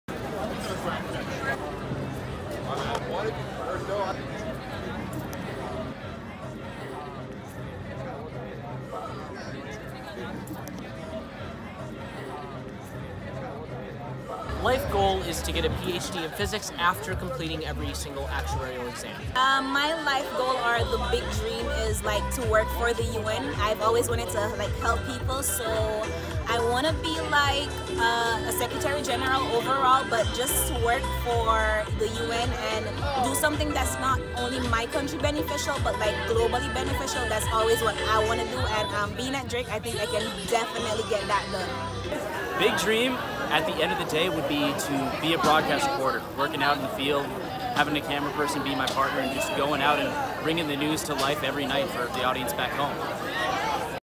Back-to-School Q&A - Drake University
Students are back in classes at Drake.  We asked what their plans are once they've completed their degree.